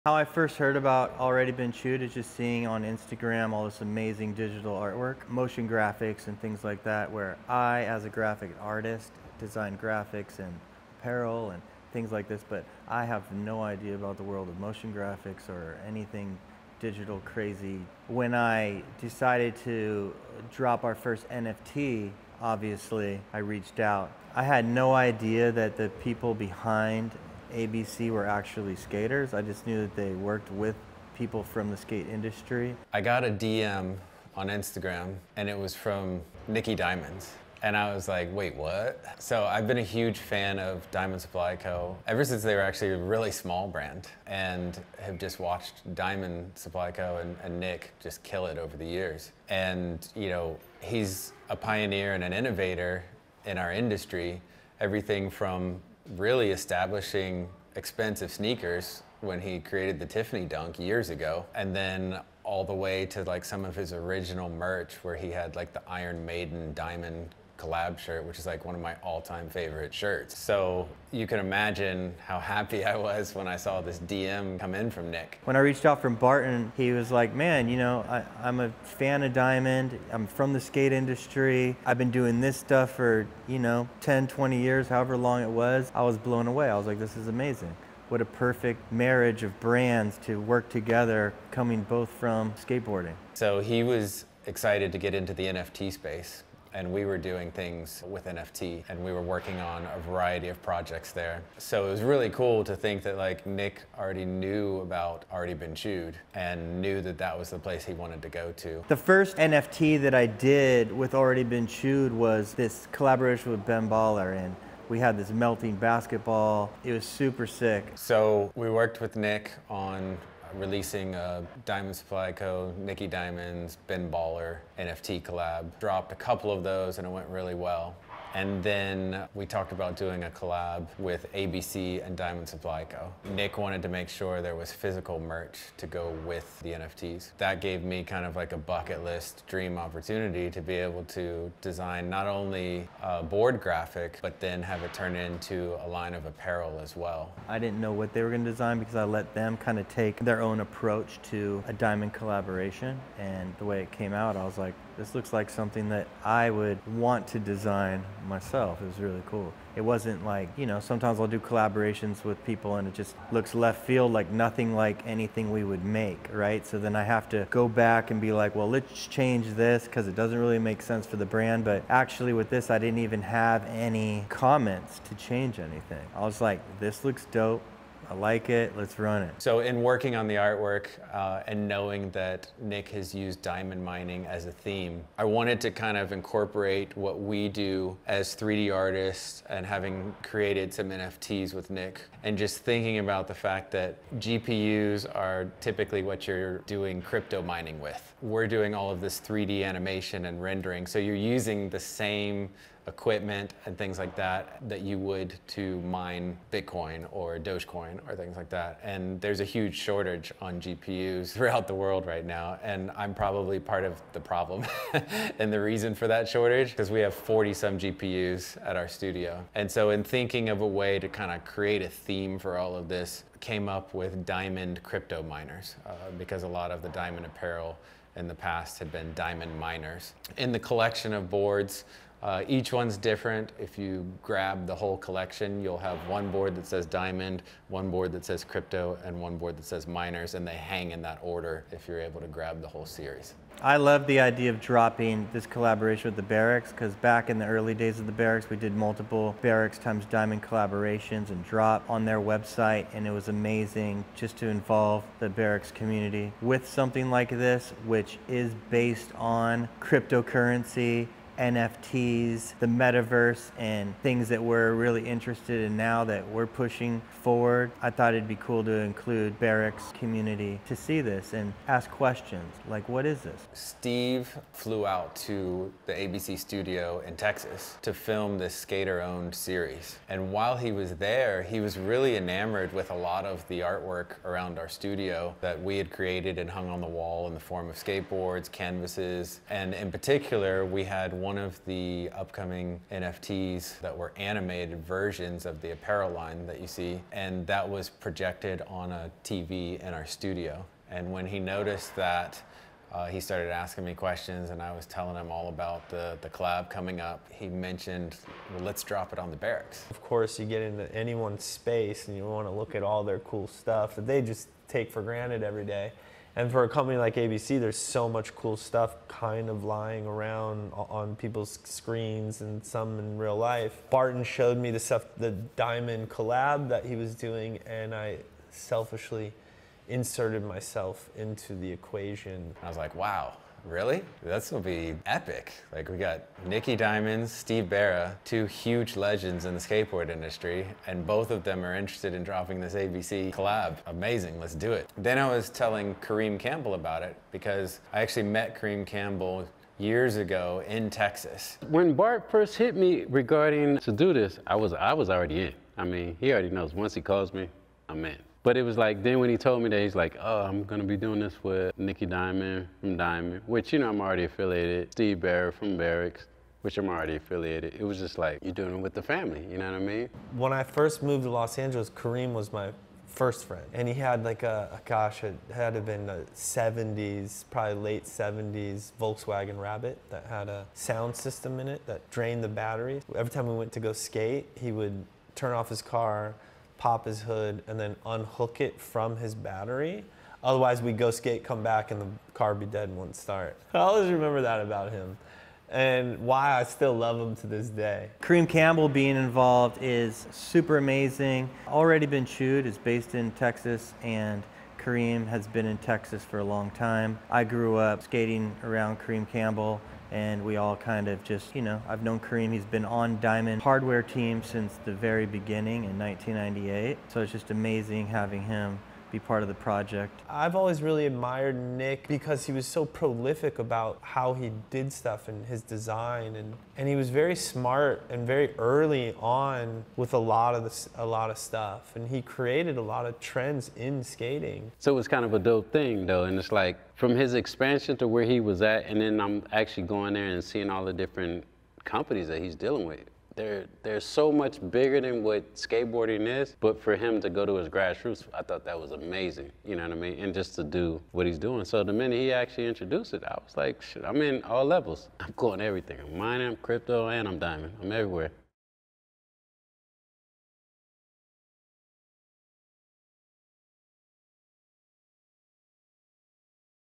THE BEST COLLAB OF 2021
Nicky Diamond, Barton Damer, Steve Berra and Kareem Campbell talk about how the new Diamond x Already Been Chewed collaboration came into existence. Dropping exclusively in The Berrics Canteen, there are only 150 signed boards, tees and hoodies.